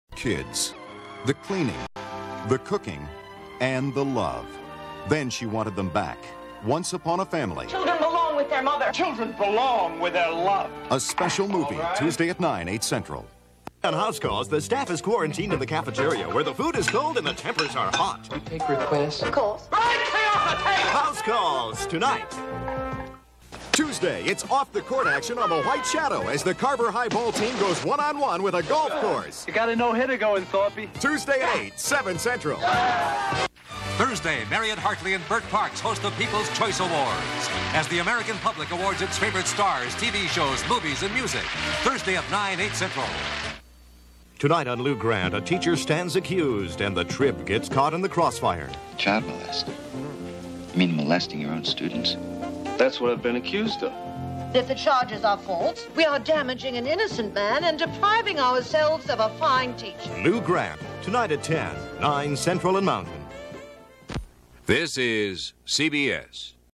Vintage CBS promos - Jan. 21, 1980
Some CBS network promos that aired on caucus night 1980. Taken from a Umatic video tape.